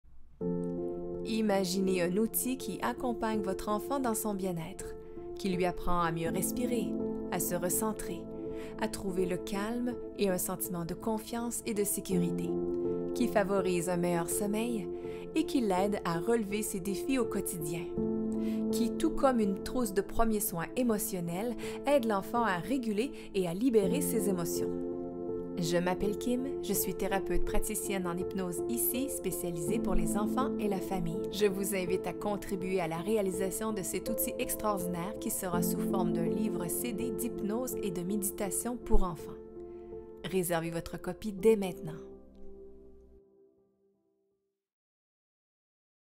Livre-CD hypnose pour enfants
Imaginez un outil qui accompagne votre enfant dans son bien-être c'est ce que le Livre-CD d'hypnose de Gribouille et Gazouillis fera dès la réalisation de ce projet en campagne de sociofinancement présentement. Réservez votre copie dès maintenant!